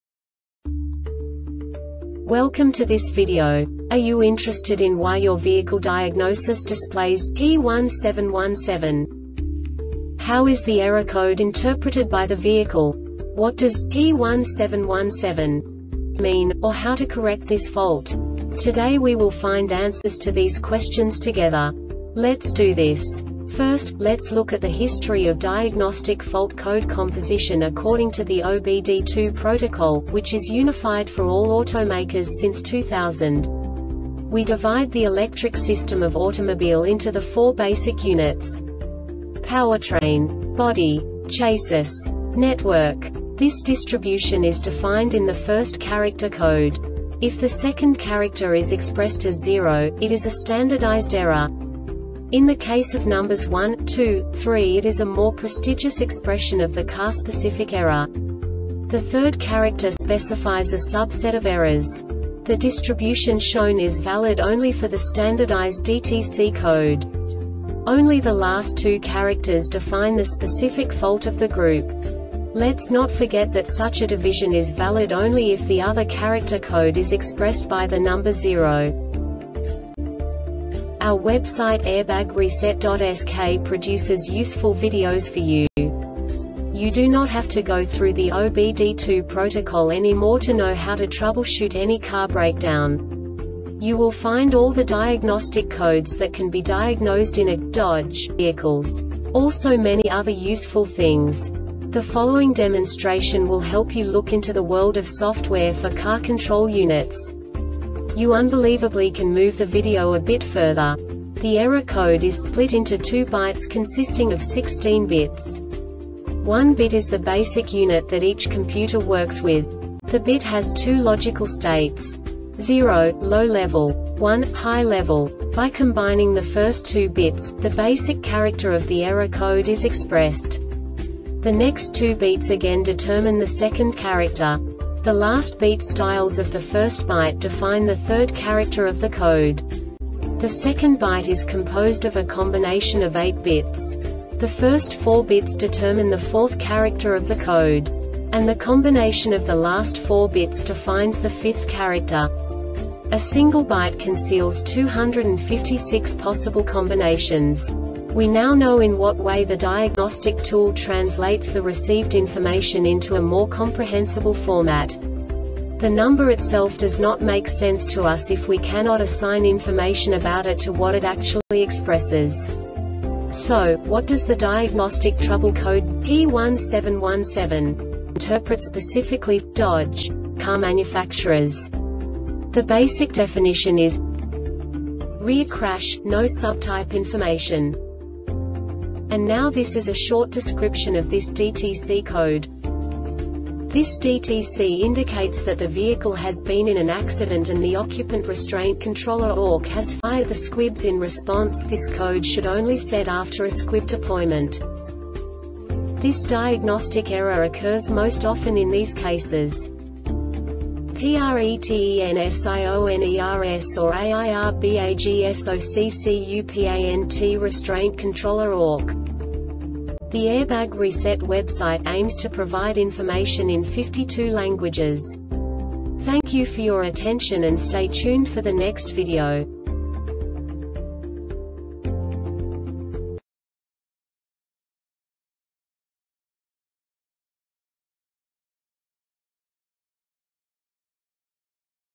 DTC Dodge P1717 Short Explanation
Contents:
0:21 Basic DTC analysis according to OBD2 protocol standard.
1:48 Insight into programming
2:58 A diagnostically understandable description of the Diagnostic Trouble Code
3:05 Basic error definition

Make:
Dodge
Code:
P1717
Definition:
Rear Crash - No Subtype Information
Description:
This DTC indicates that the vehicle has been in an accident and the Occupant Restraint Controller (ORC) has fired the squibs in response. This code should only set after a squib deployment.
Cause:
PRETENSIONERS or AIRBAGSOCCUPANT RESTRAINT CONTROLLER (ORC)